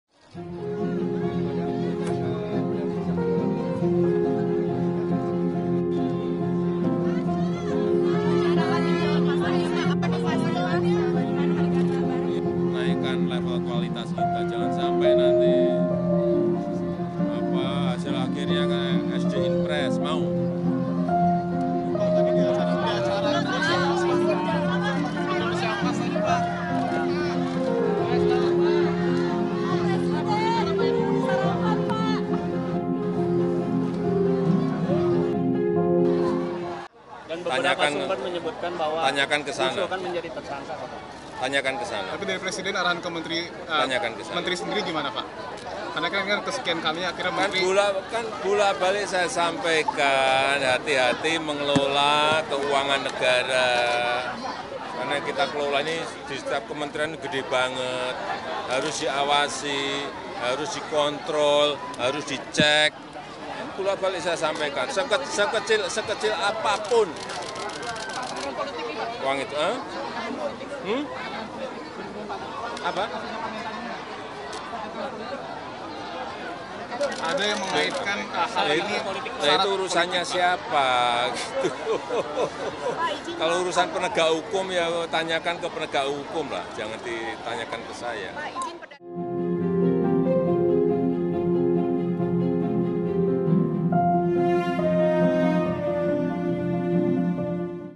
Isu Mentan Tersangka KPK, Jokowi: Bolak-balik Saya Sampaikan
Presiden Joko Widodo mewanti-wanti agar dalam pengelolaan keuangan di Kementerian dan Lembaga tidak terjadi kesalahan penggunaan yang berujung tindak pidana korupsi.

Hal ini merupakan respons terhadap informasi tentang akan adanya penetapan tersangka kasus korupsi Menteri Pertanian oleh Komisi Pemberantasan Korupsi.